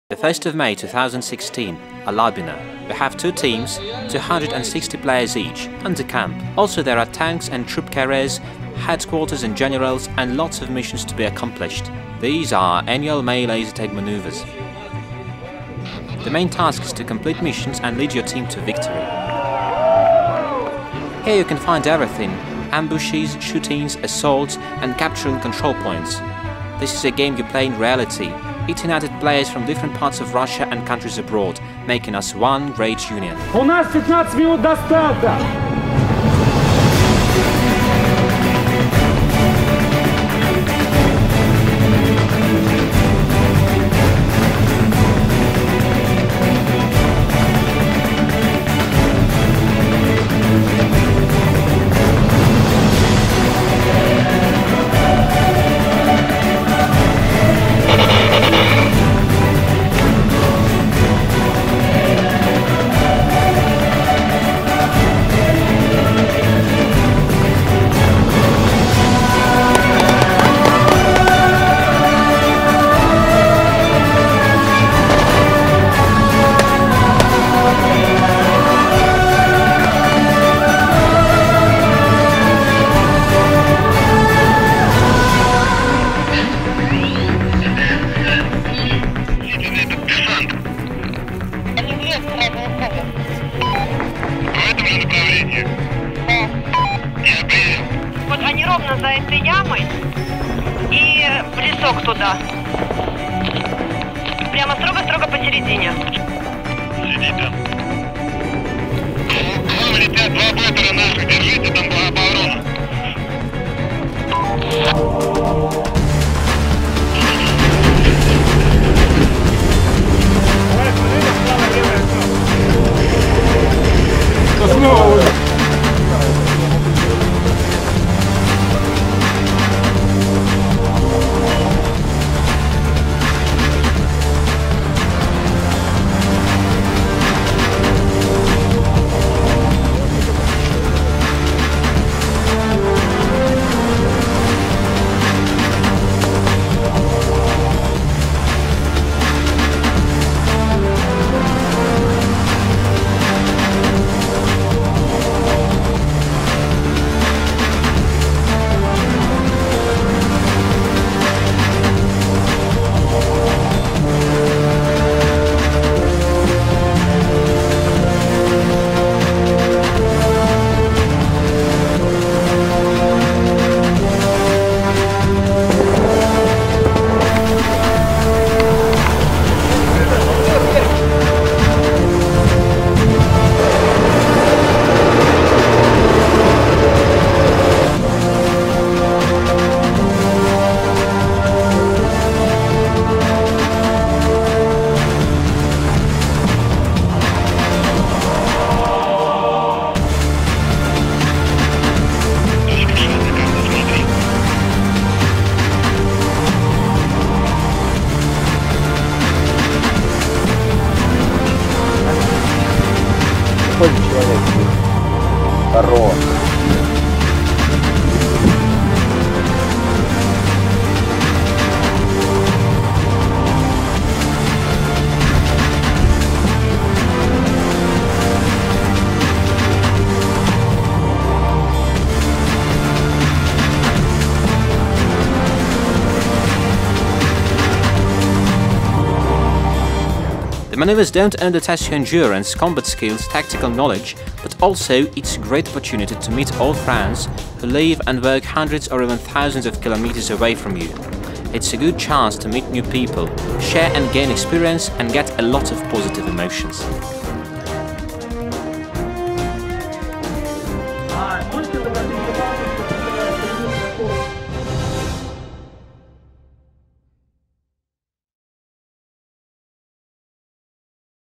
The biggest militrary laser tag battle in 2016 (Alabino)
Tactical Laser tag battle with tanks and wheeled troop transporters